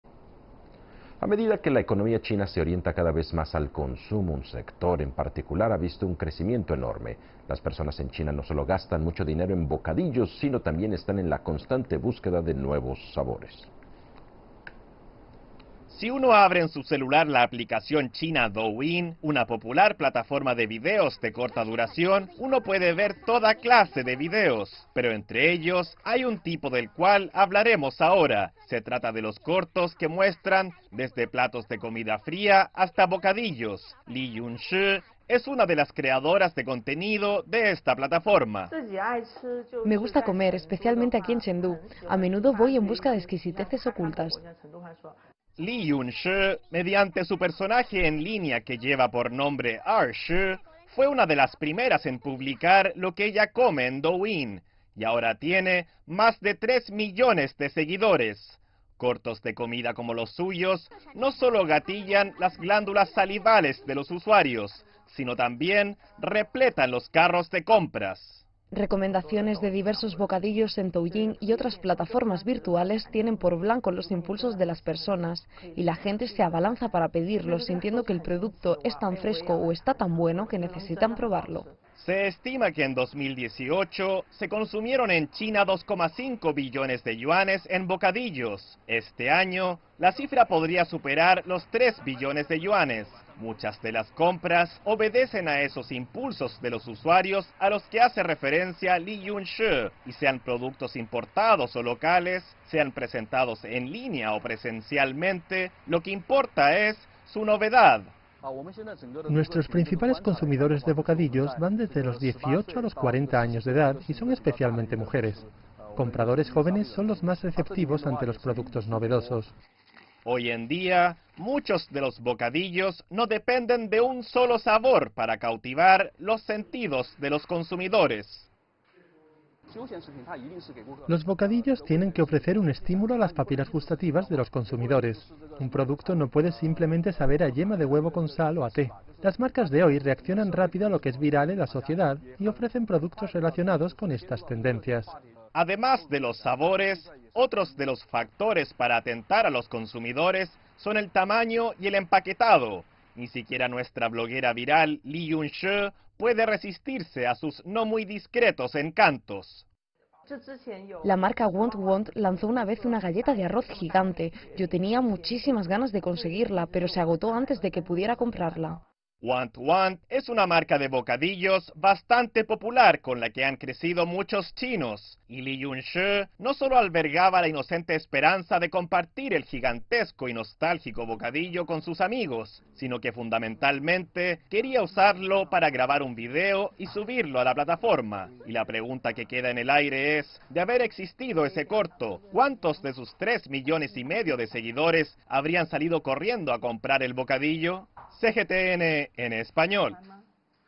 El consumo chino de bocadillos se aproxima a los 3 millones de yuanes
A medida que la economía china se orienta cada vez más al consumo, un sector en particular ha visto un crecimiento enorme. Las personas en China no sólo gastan mucho dinero en bocadillos, sino también están en la constante búsqueda de nuevos sabores.